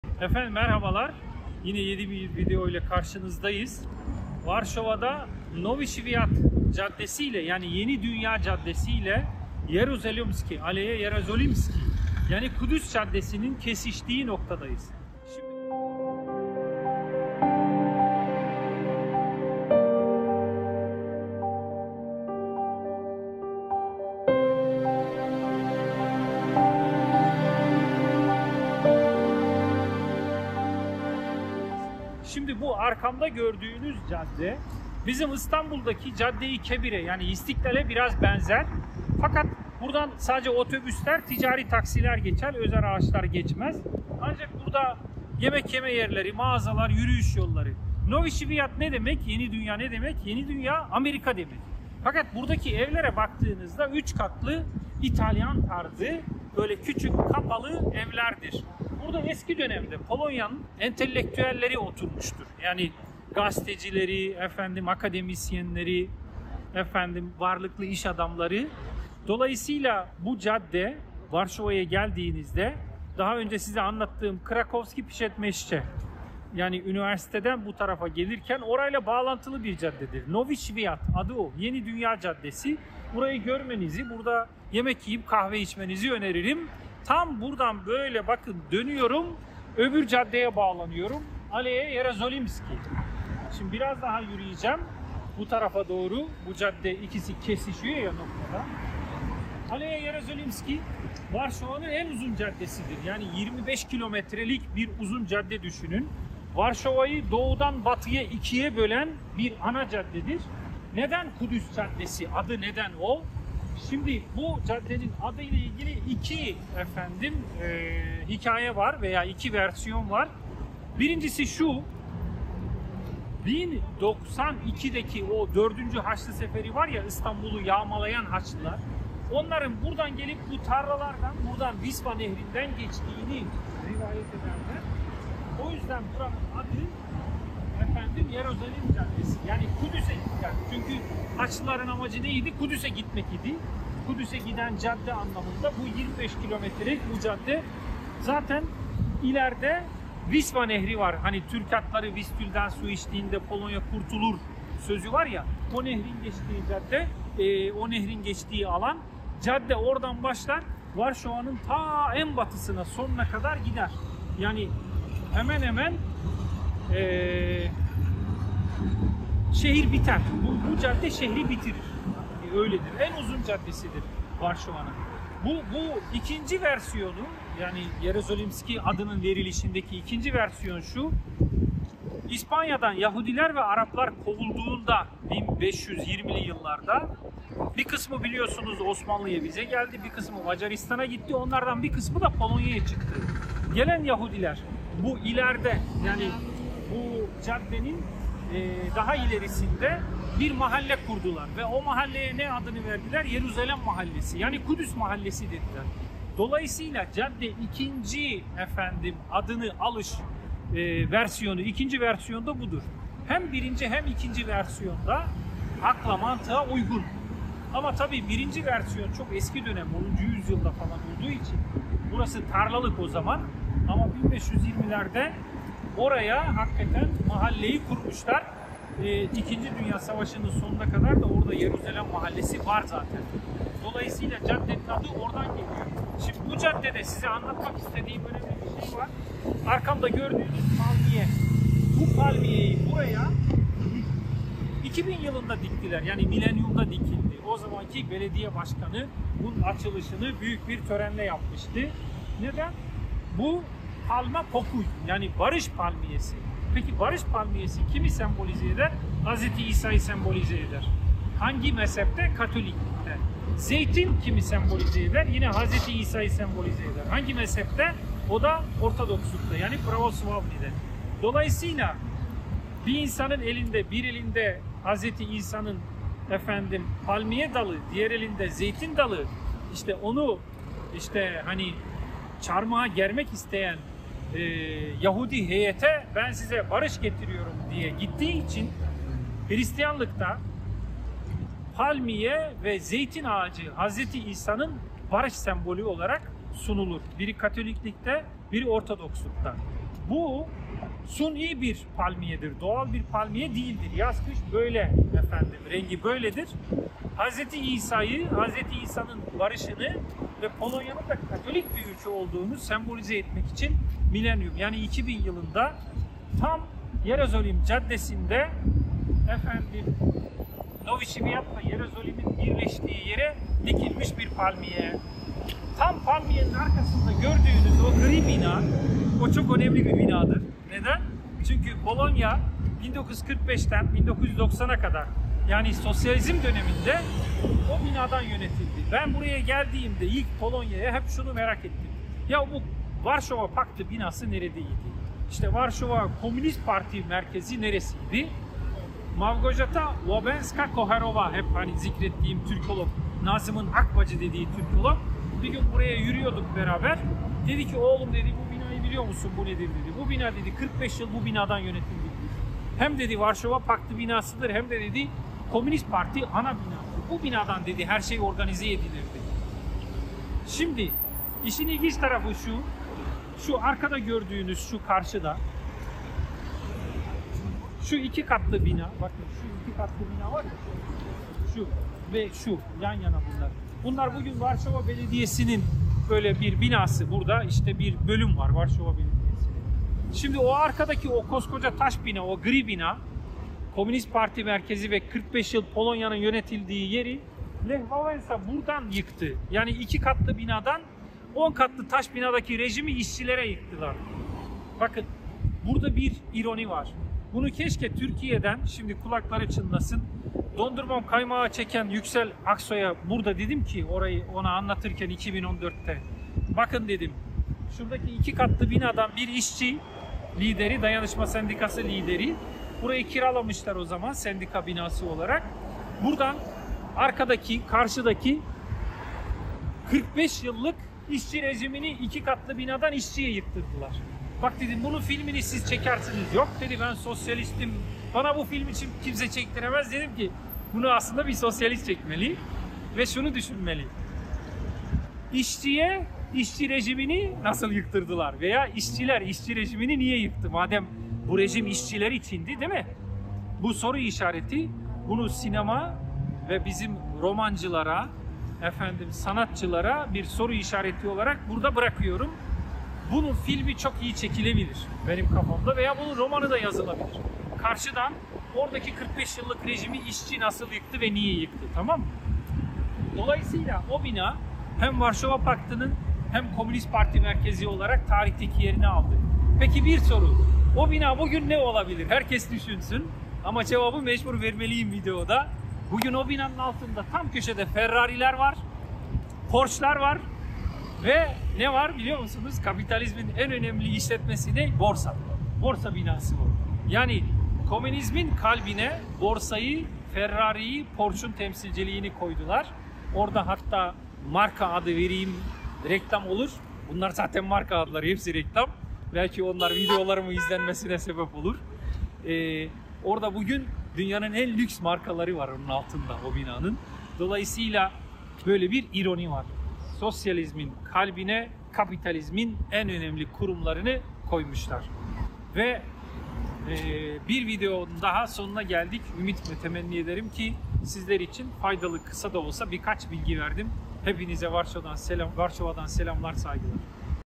Varşova’da Yeni Dünya ve Kudüs Caddeleri öztürk emiroğlu aleja jerozolimskie
Varşova 10. yüzyılda kurulmuş bir şehirdir. Polonya Devlet tarihinde Gniezno ve Krakov’dan sonra üçüncü başkenttir. Varşova’nın en eski semti eski şehir kısmı, en uzun caddesi ise Jerozolimskie’dir. Jerozolimski yani Kudüs caddesi Varşova’yı doğudan batıya ikiye böler. Cadde Vistül nehri tarafından başlar ve Pruszkow tarafında biter. Tarihi değeri olan bu caddeye bağlanan caddelerden biri Nowy Swiat yani Yeni Dünya Caddesidir.